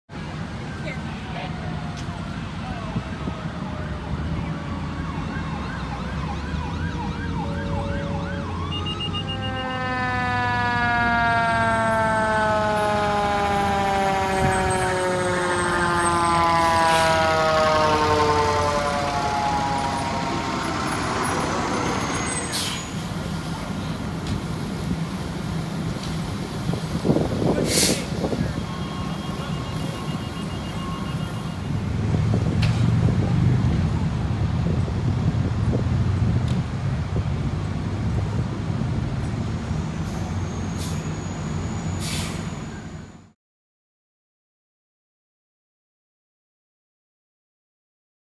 *On Scene* Queensbury Central Fire, Working Vehicle Fire, 8-20-19
At approximately 14:35, Queensbury Central Fire was dispatched to a vehicle fire endangering the structure, which was soon confirmed by Engine Rescue 316 and patrol. West Glens Falls Fire was requested mutual aid to cover the QCFD district, as they were at this vehicle fire and a mutual aid structure fire in Washington County.